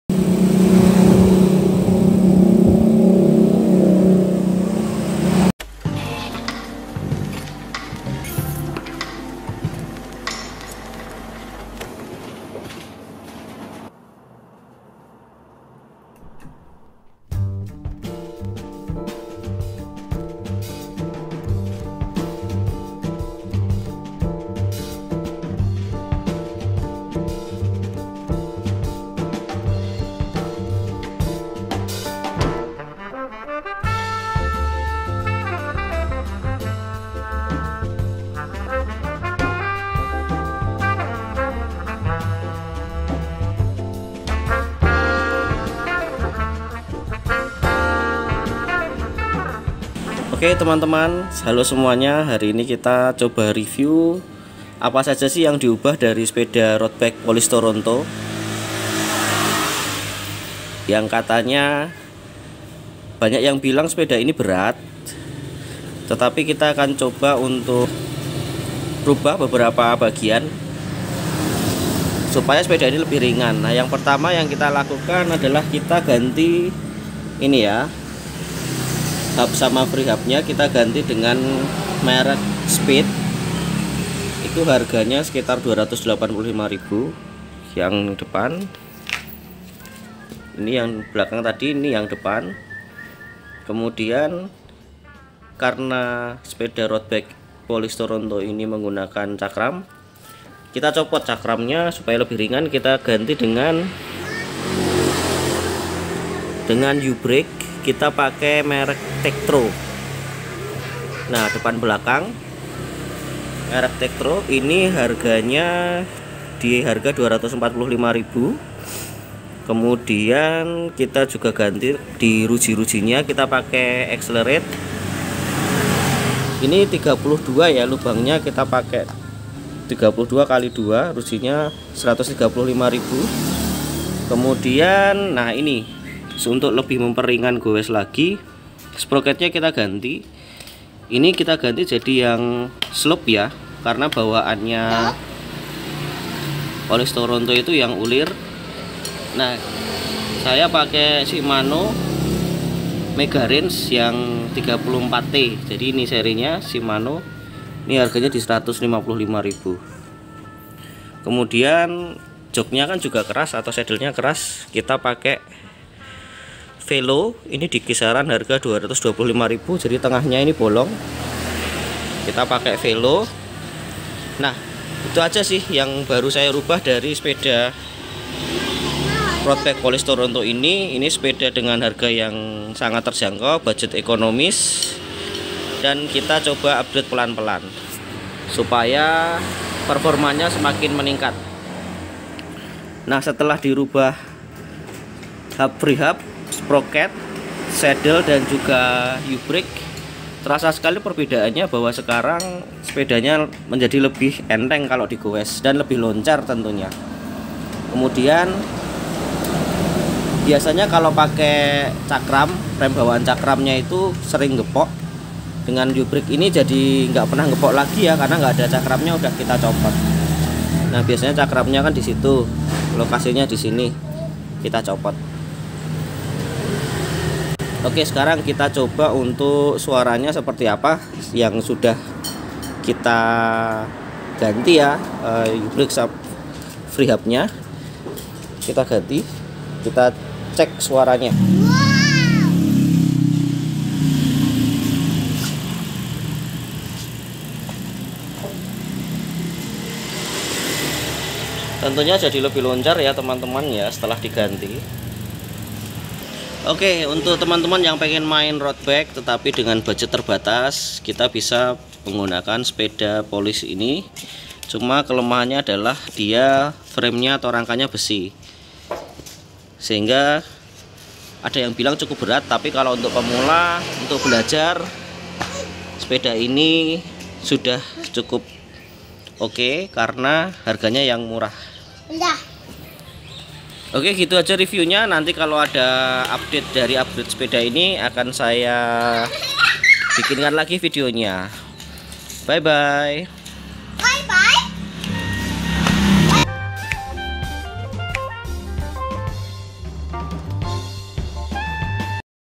Upgrade Police Toronto
Halo semua, hari ini kita buat video tentang upgrade yang saya lakukan pada sepeda roadbike police toronto. Sepeda ini merupakan roadbike dengan frame besi yang harganya sangat terjangkau.
Memang sepeda ini berat, sehingga kita perlu lakukan upgrade bertahap agar makin loncer dan gowesnya semakin nyaman.

Berikut upgrade yang sudah saya lakukan:
- sadel (merk Velo)
- hub freehub (merk Speed)
- rem u brake (merk Trekto)
- ruji (merk XLR8)
- rantai (merk Prompt)
- sproket (merk Shimano 11-34 T Megarange)
- rim tape ban